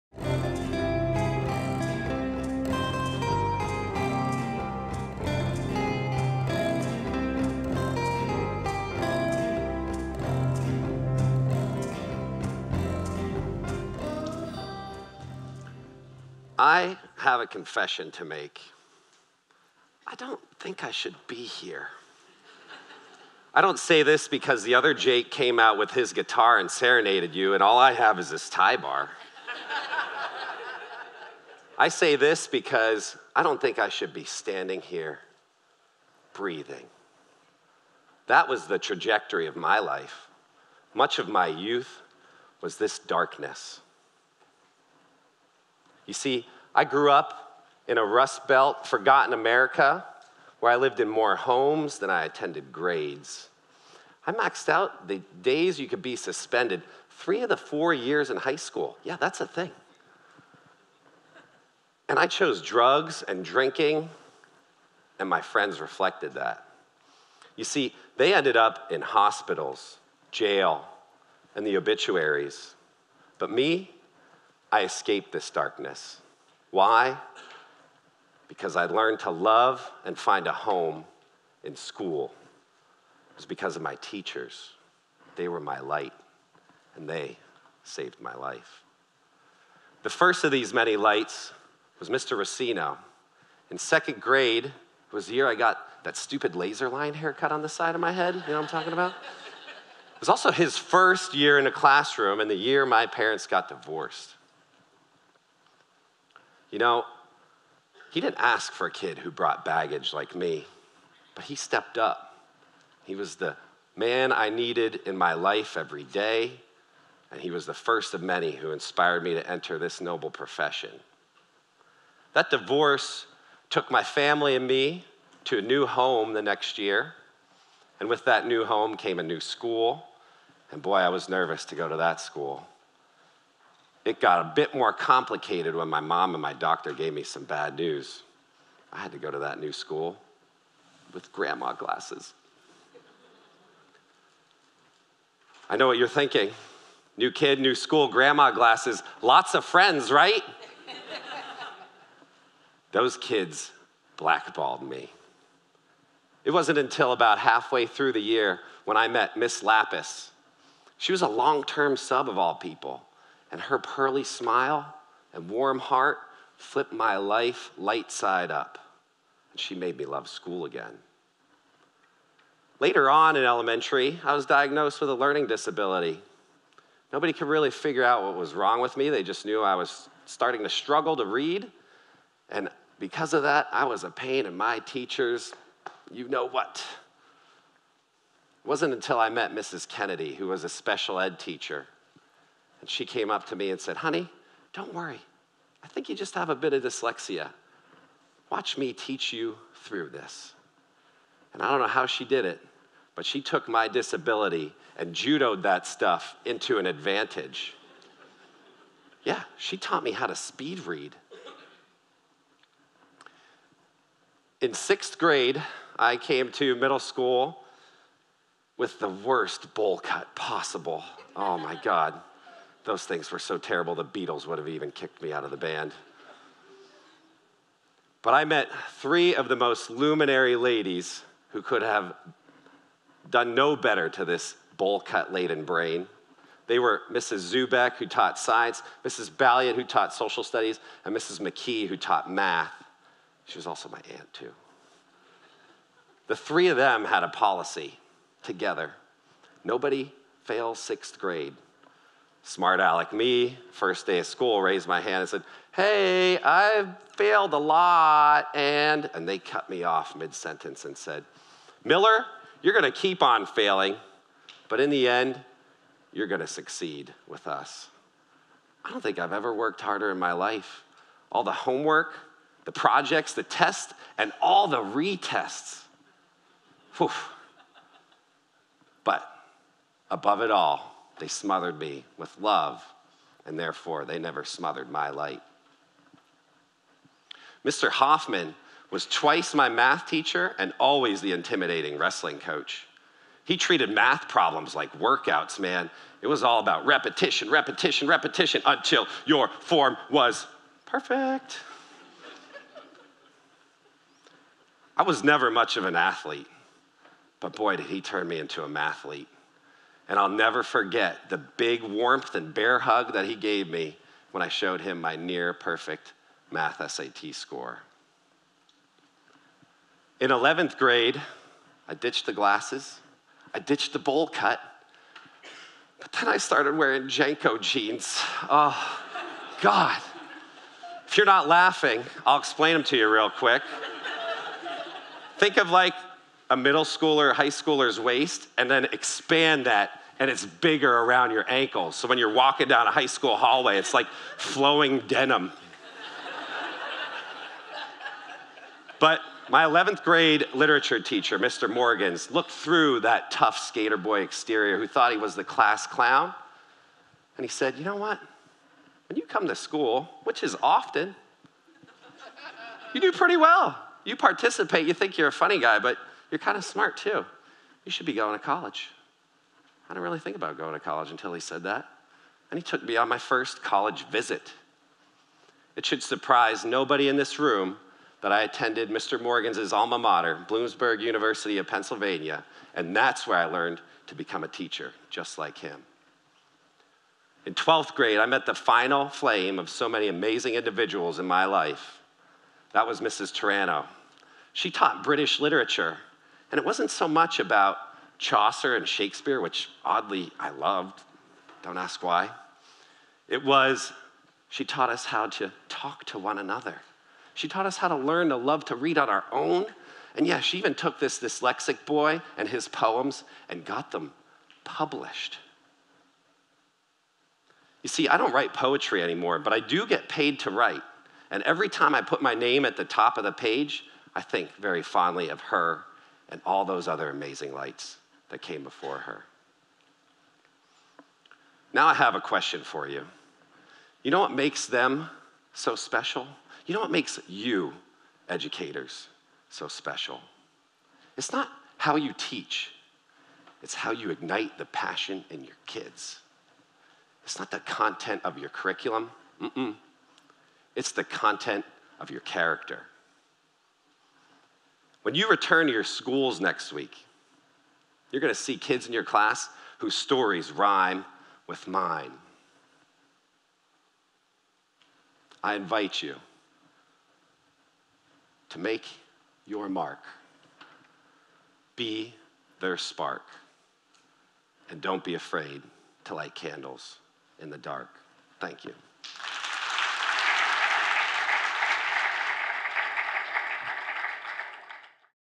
How My Teachers Saved My Life
Jake Miller's story from the Keeping the Promise of Public Education Symposium.